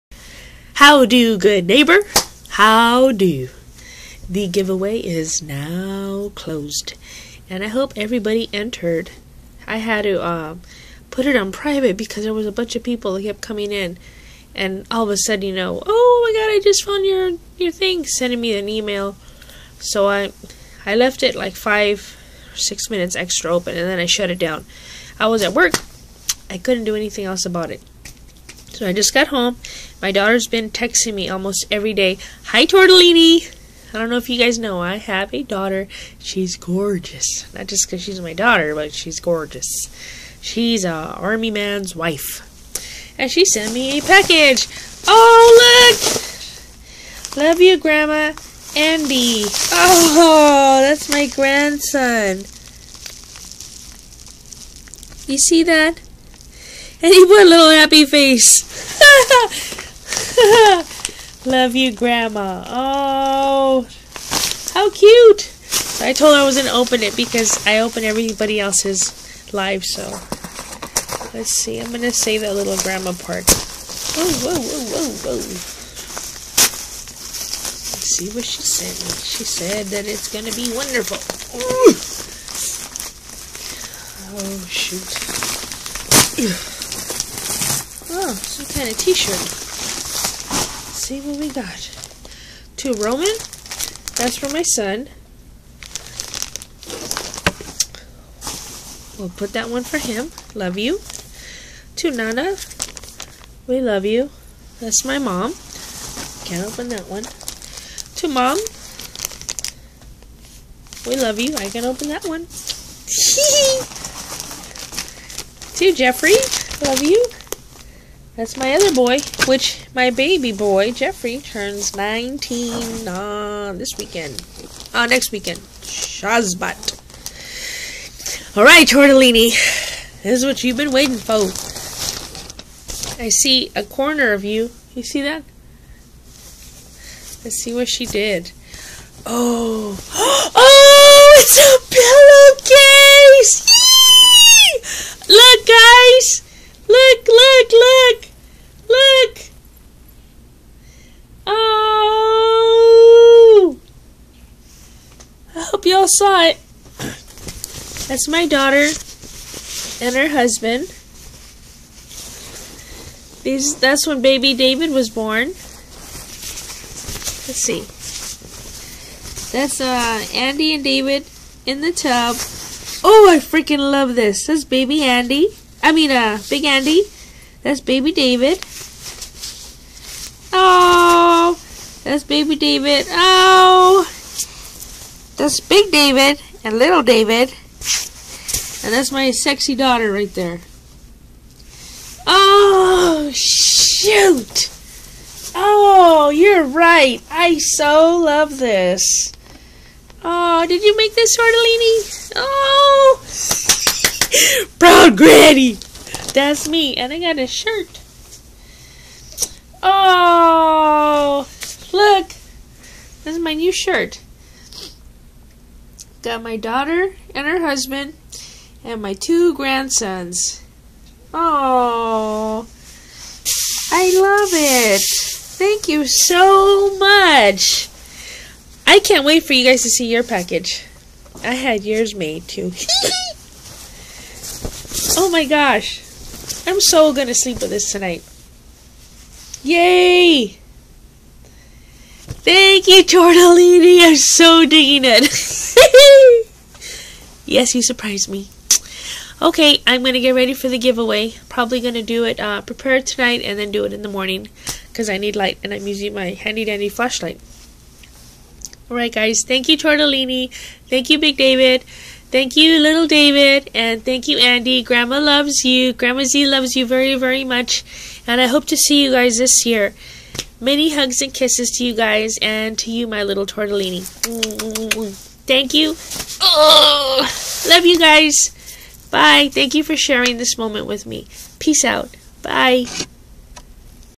Thank you Tortellini :)
My daughter said she made me a few things.. I frikken love them! Thank you so much!!